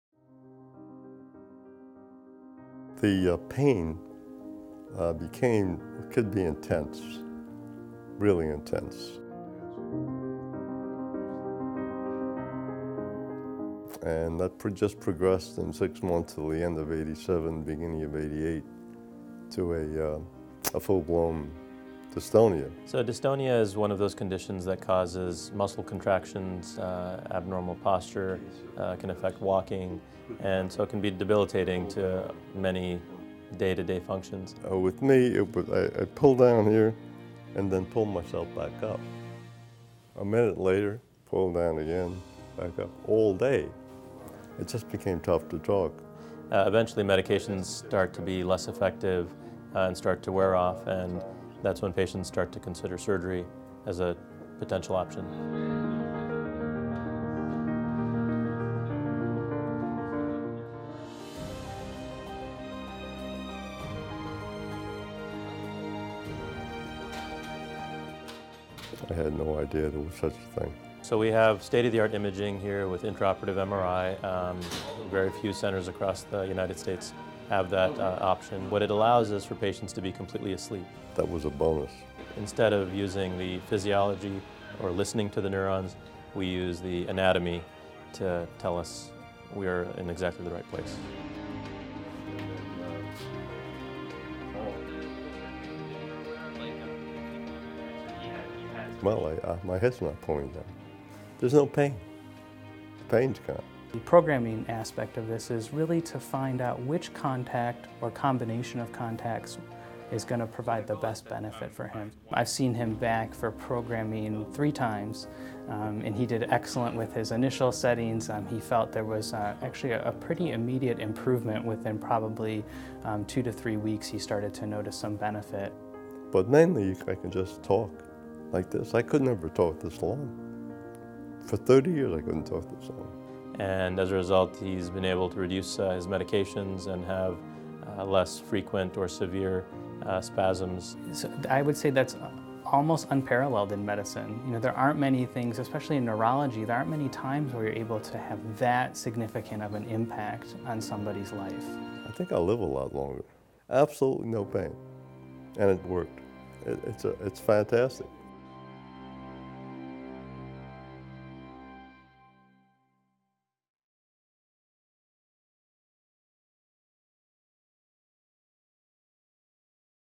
DBS surgery offers relief from dystonia | Duke Health
New technology at Duke allows patients with movement disorders to have deep brain stimulation surgery while asleep. Hear from one dystonia patient from Raleigh about how the surgery has improved his quality of life.